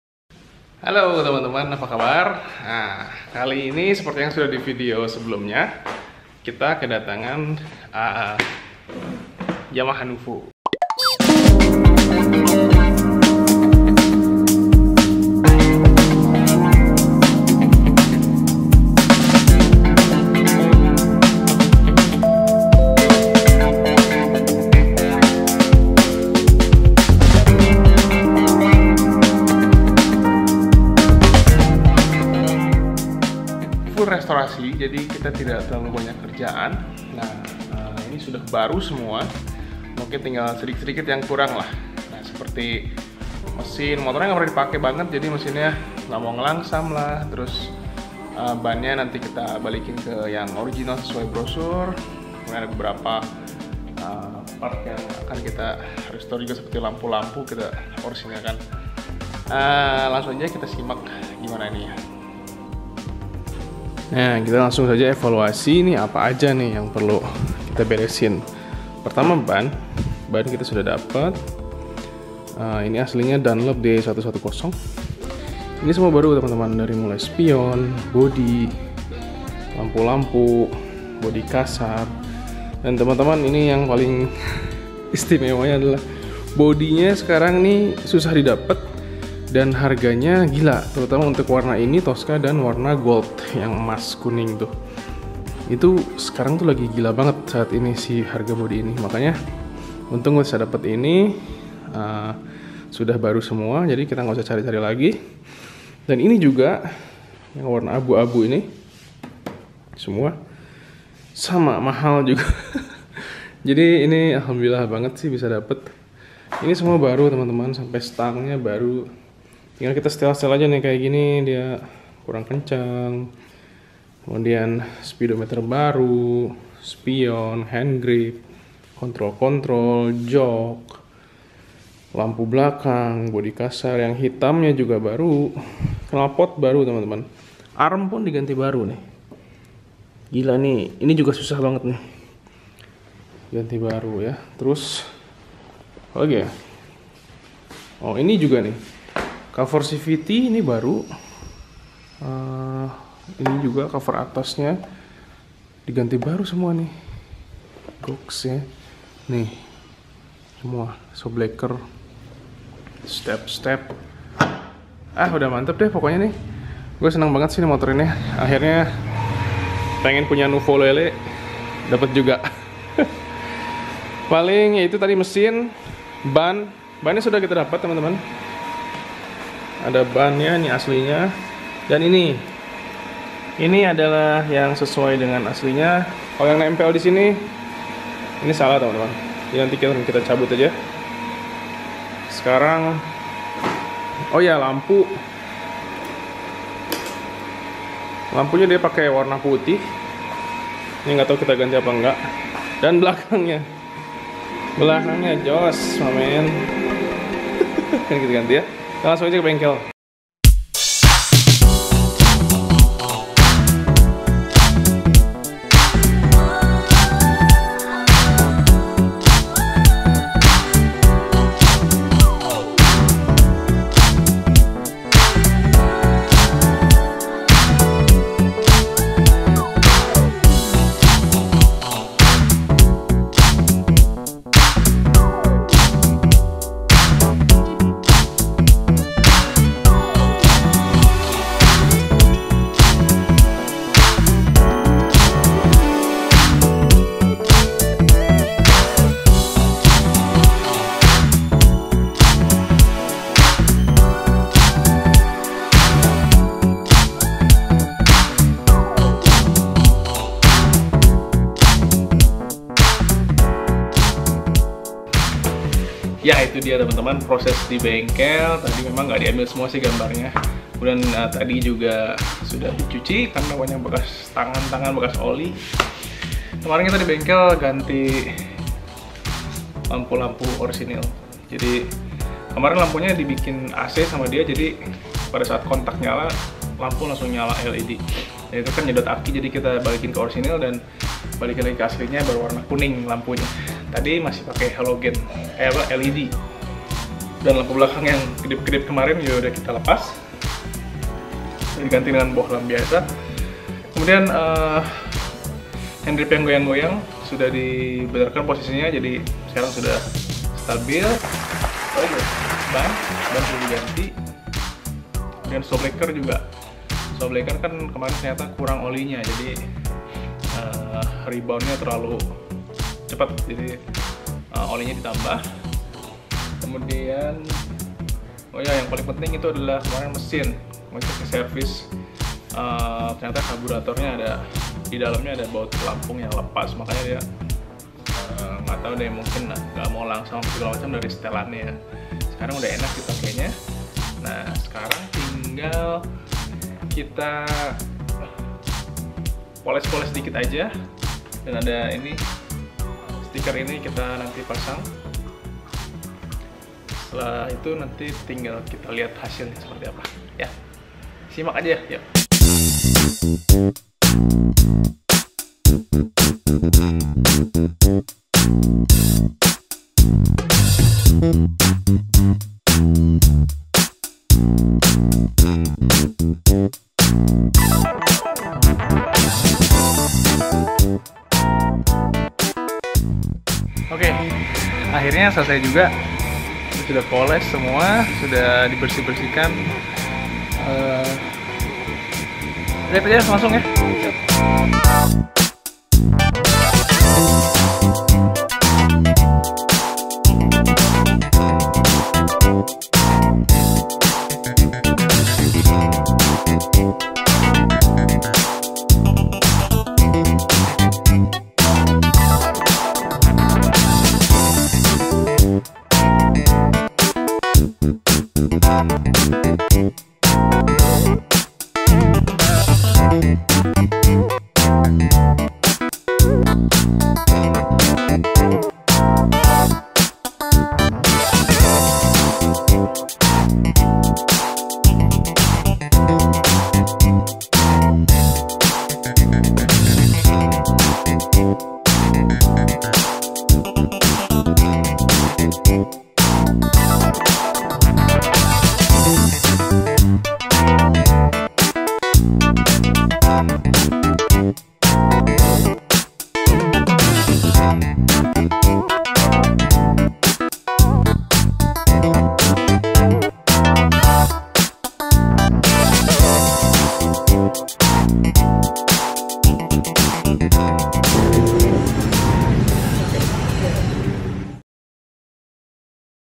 Dapet Yamaha Nouvo Full restorasi, warna kesukaan mirip mini 1m [Garage For Lyfe] ACB Vlog Eps-30
Halo teman2.. maaf lama gak upload..
Kali ini video lanjutan dari dua episode sebelumnya.. tonton ya..

Selamat Menonton.. Terimakasih 🙏